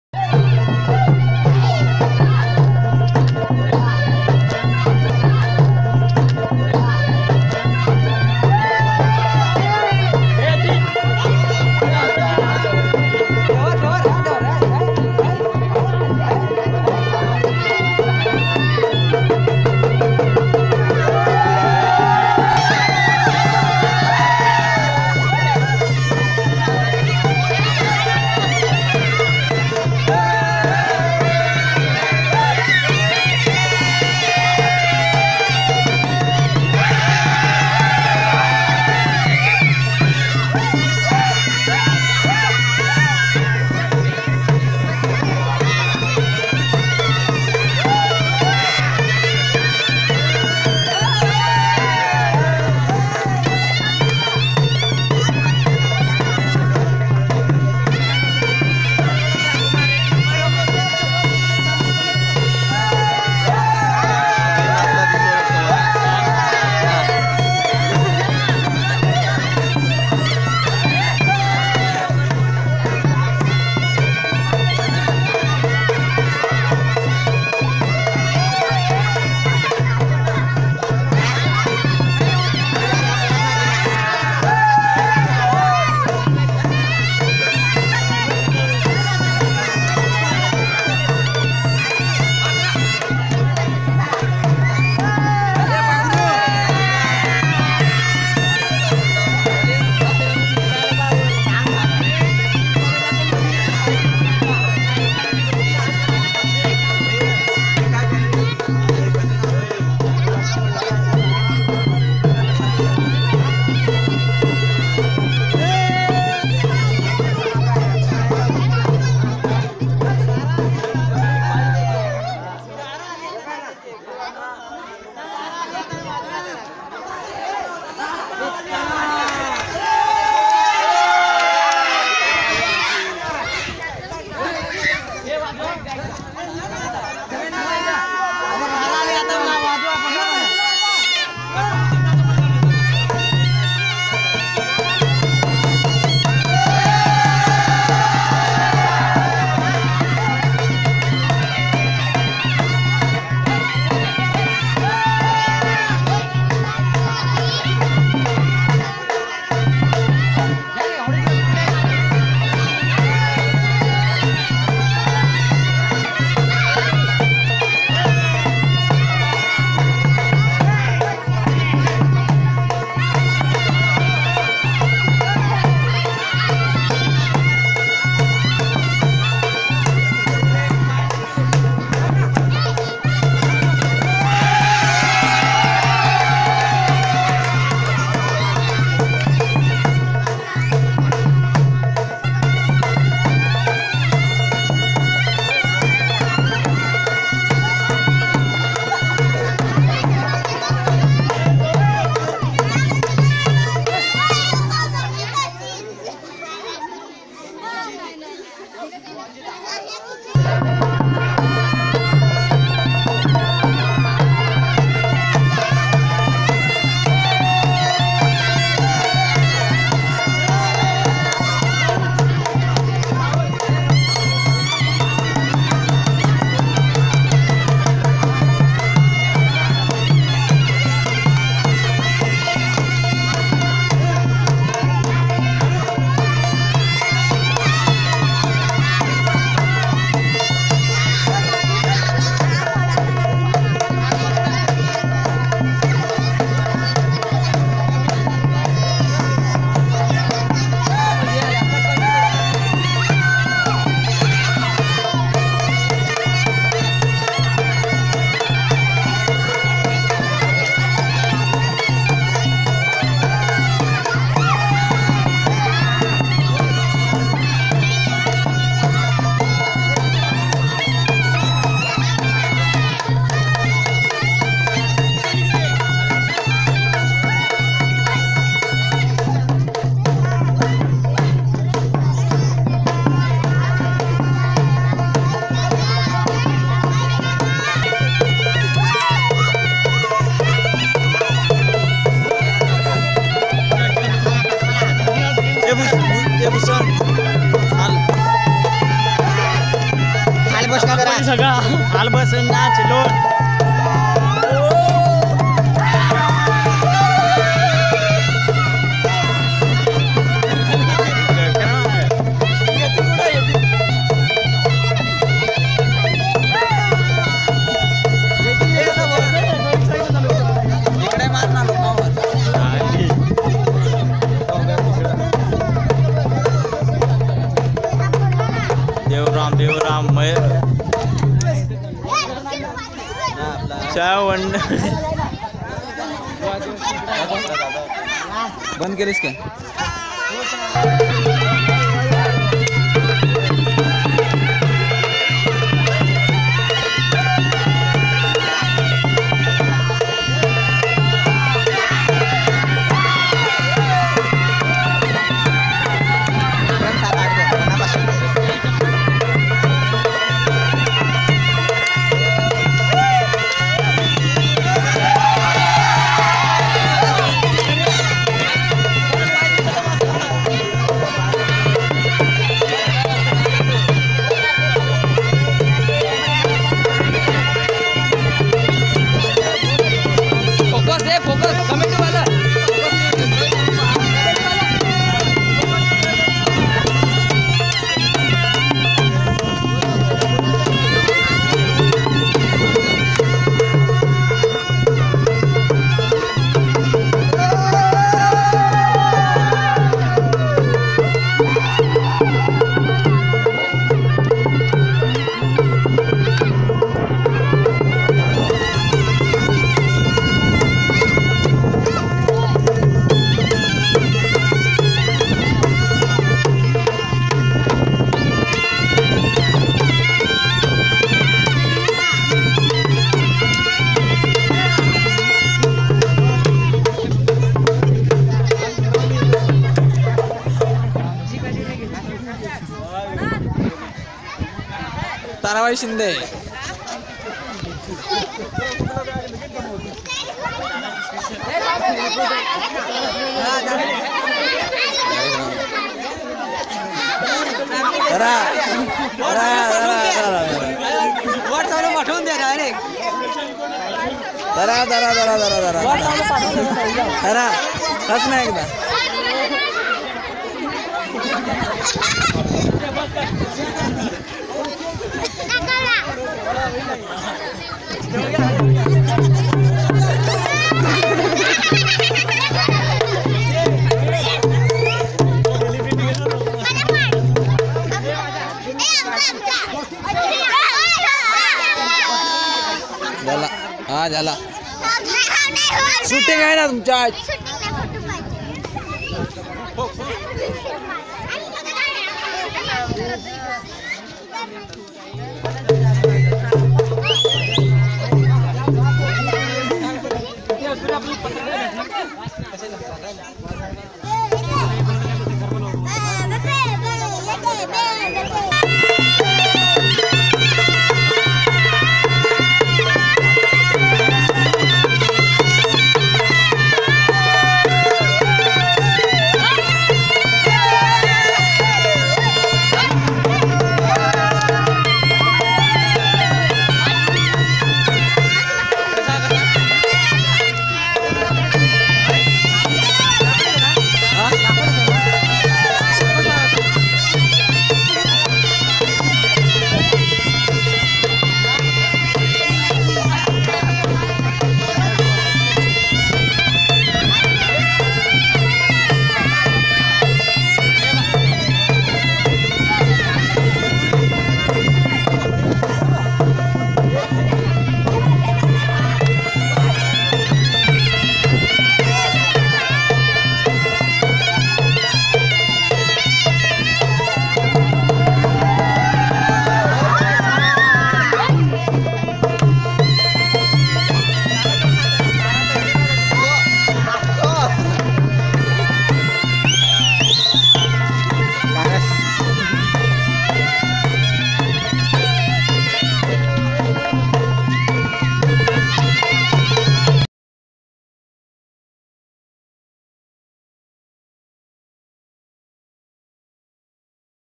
Morya Group Dahihandi Festival 2016- kogade-jawhar आदिवासी तारपा/सांबळ नृत्य
AADIVASI LIFE STYLE DANCE आदिवासी सांबळां नृत्य